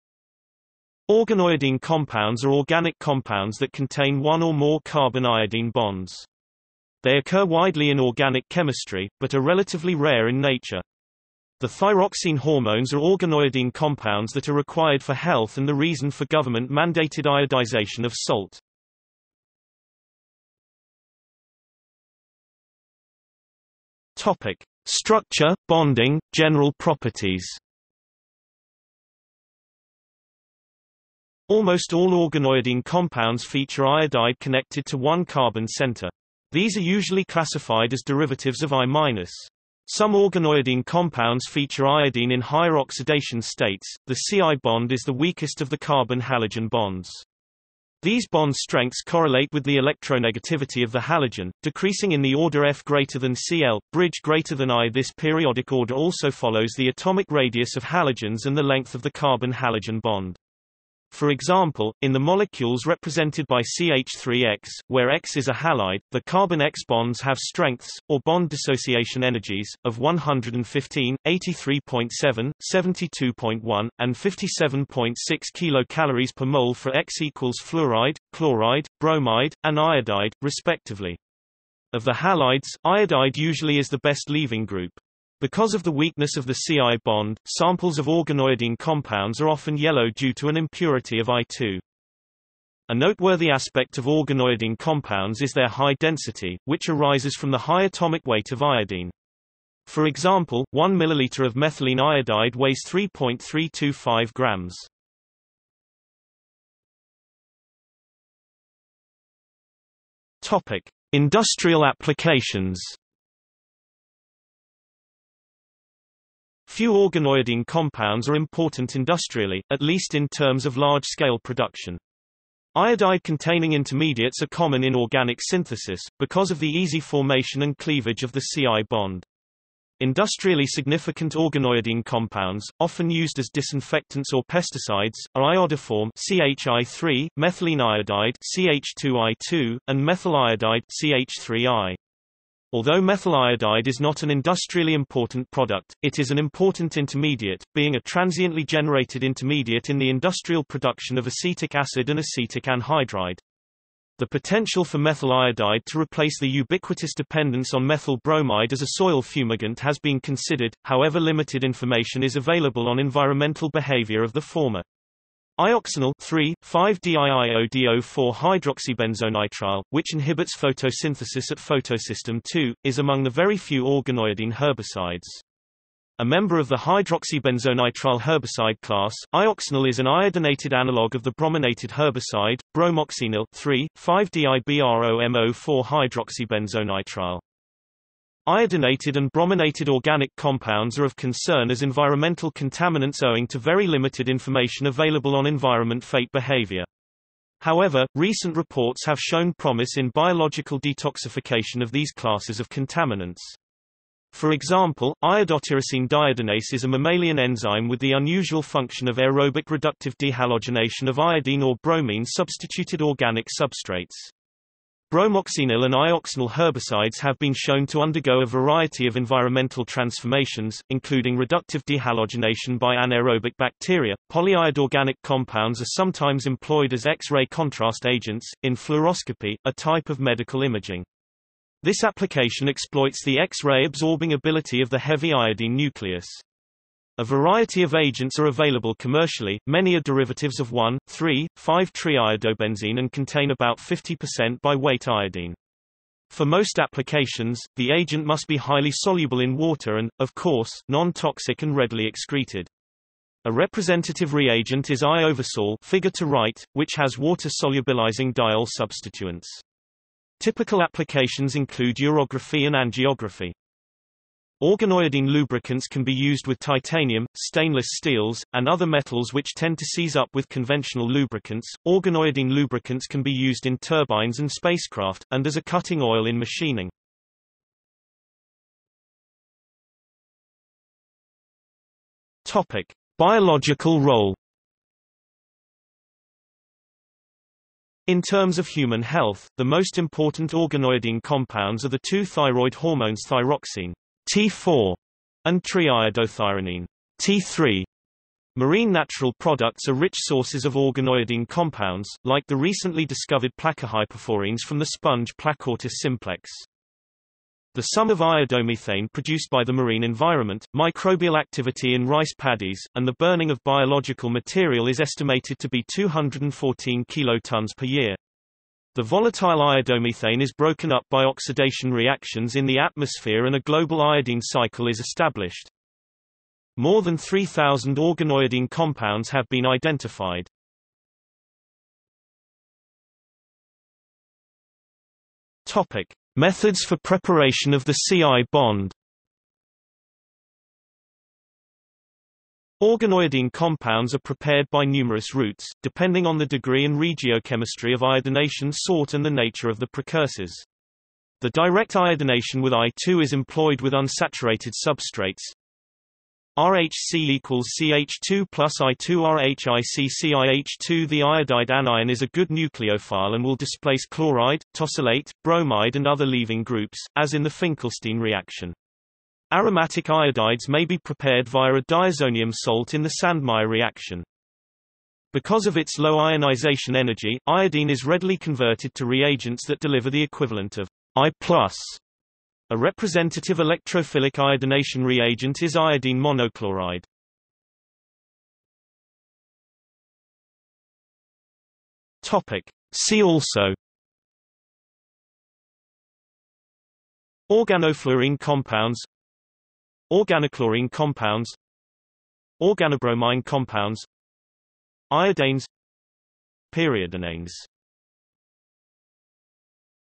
Organoiodine chemistry | Wikipedia audio article
00:00:20 1 Structure, bonding, general properties
00:01:46 2 Industrial applications
00:04:38 3 Biological role
00:05:32 4 Methods for preparation of the C&#8211;I bond
00:06:28 5 See also

- Socrates

SUMMARY
Organoiodine compounds are organic compounds that contain one or more carbon–iodine bonds.  They occur widely in organic chemistry, but are relatively rare in nature.  The thyroxine hormones are organoiodine compounds that are required for health and the reason for government-mandated iodization of salt.